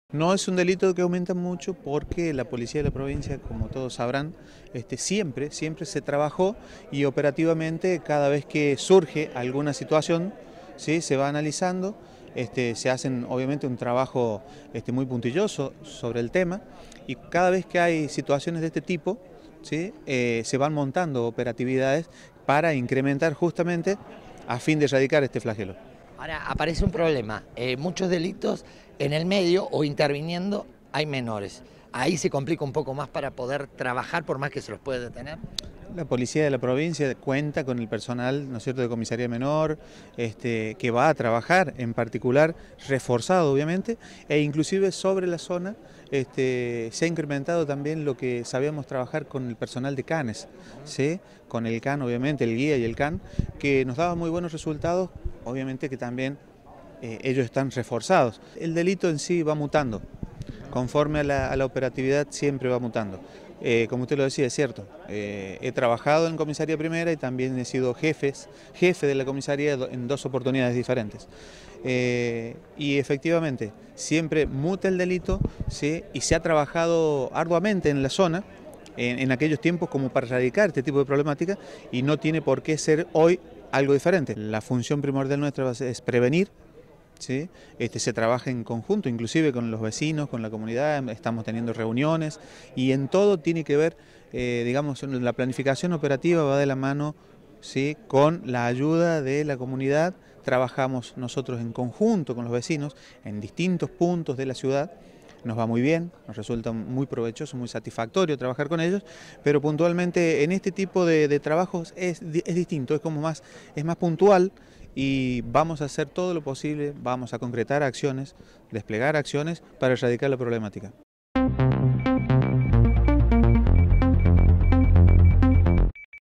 05-06-2019/// LA DIVISIÓN CANES SE SUMARÁN A LOS CONTROLES DE SEGURIDAD EN EL CENTRO
CRÍO DARÍO NEIRA - SUB JEFE POLICÍA.-
EL SUBJEFE DE POLICÍA ANUNCIÓ NUEVAS MEDIDAS EN TORNO A LOS ROBOS DE MECHERAS EN LOS COMERCIOS DEL CENTRO. DESDE HACE UNOS DÍAS HAY PERSONAL POLICIAL CON PERROS RECORRIENDO ESTA JURISDICCIÓN EN LOS PRÓXIMOS DIAS SE PODRÁN VER OTRAS NOVEDADES. CON RESPECTO A LA UTILIZACIÓN DE MENORES PARA DELINQUIR EL COMISARIO NEIRA MANIFIESTA, QUE SE HACEN LAS ACTUACIONES CORRESPONDIENTES.-